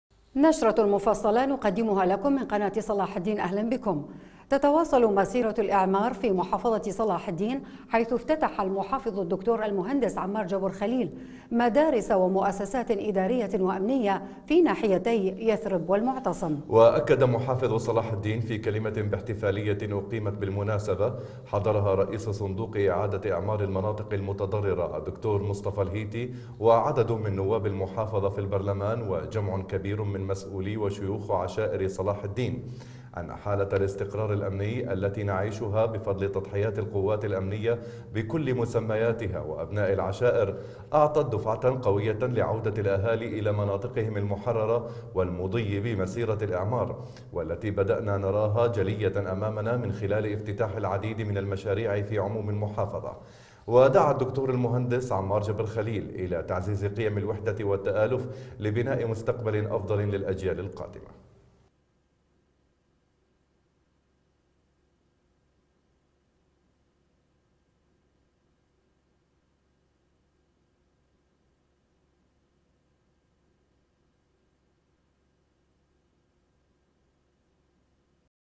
العناوين
محافظ صلاح الدين ورئيس صندوق اعادة الاعمار يفتتحان العديد من المدارس والدوائر الامنية والخدمية في يثرب والمعتصم

محافظ صلاح الدين يعد بناء الانسان اقصر الطرق لاقتلاع ثقافة داعش الارهابي ويؤكد ان الانتماء للعراق لا بديل له

سفر الرئيس صالح الى دافوس يؤجل اختيار خليفة عبد المهدي والخلاف على المرشحين يدخل مرحلة معقدة

التربية تنفي تأجيل العام الدراسي الحالي والداخلية تنجح باغلاق مئات موقع داعش للحرب النفسية وخلط الاوراق الامنية